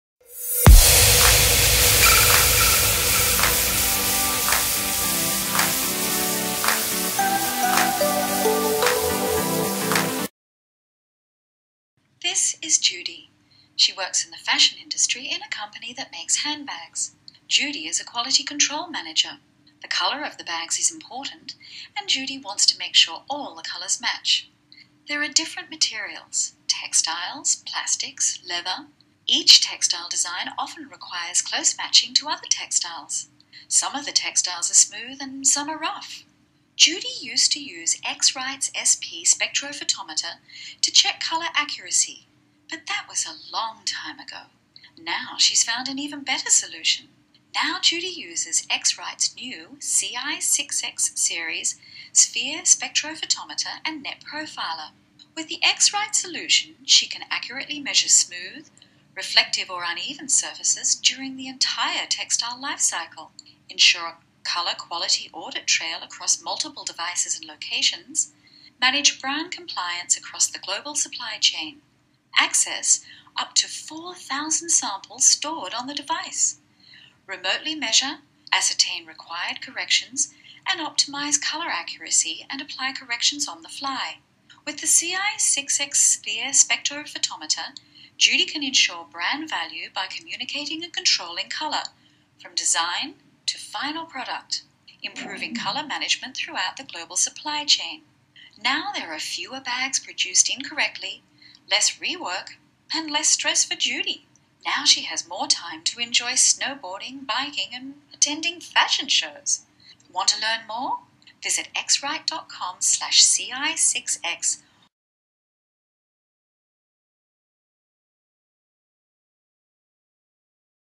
X-Rite Ci6x Series Portable Spectrophotometers
When colors errors occur, whether there is confusion between customer specifications and production output or batch-to-batch color variation, the result is costly waste and rework. The X-Rite Ci6x family of sphere spectrophotometers, together with NetProfiler, Color iMatch and Color iQC software, reduce rejection rates and scrap and improve time to market and profitability. With a variety of configurations and price points, the Ci6x family offers solutions for organizations of all types and sizes.